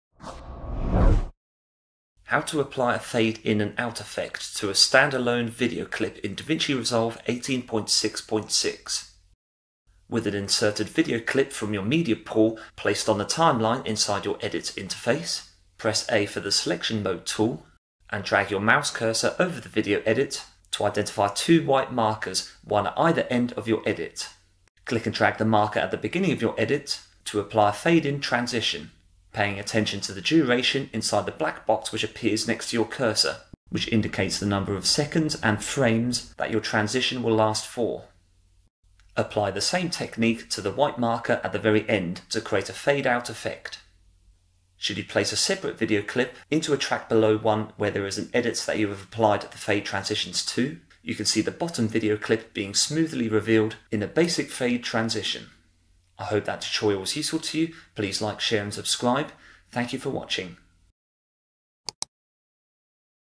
DaVinci Resolve Tutorial: How to Create a Basic Fade In/Out Video Transition
DaVinci Resolve Tutorial #225. In this video, you will see how to manually apply a fade in/out effect to the start and end of a video clip edit in version 18.6.6 of the software.

***SPECIAL OFFERS

3) VISUALVFX

C0NTI